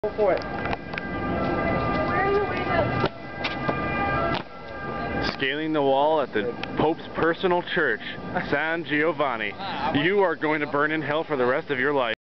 Europe Trip 2007 KA
Urban Scailing the wall of the basilica in Rome